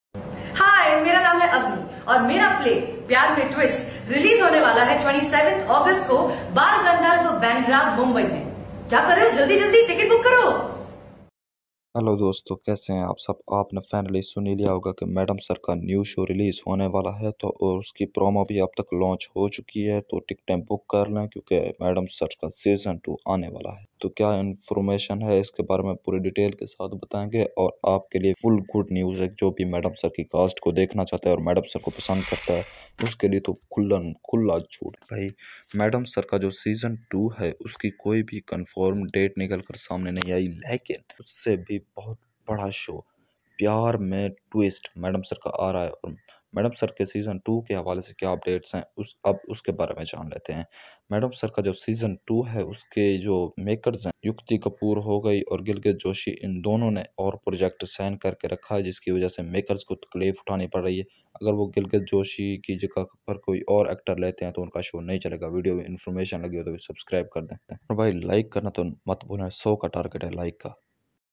Madim sir season 2 big update || gulke Joshi New show promo || Tariq nazeer official
gulke Joshi New show main kam Kar rahi hai aur vah shok kis diamond wala aur Kya hai iski vajah hogi purushon mein kaun kaun si cast dekhne ko milegi yah story tahsil mein aaj kis video mein Bata rakhi